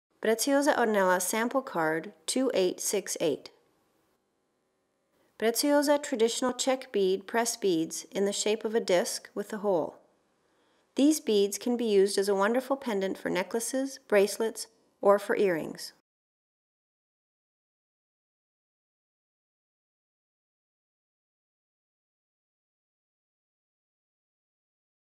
PRECIOSA ORNELA SAMPLE CARD 2868
Preciosa Traditional Czech Bead pressed beads in the shape of a disc, with one hole.  Beads can be used as a wonderful pendant for necklaces, bracelets, or for earrings.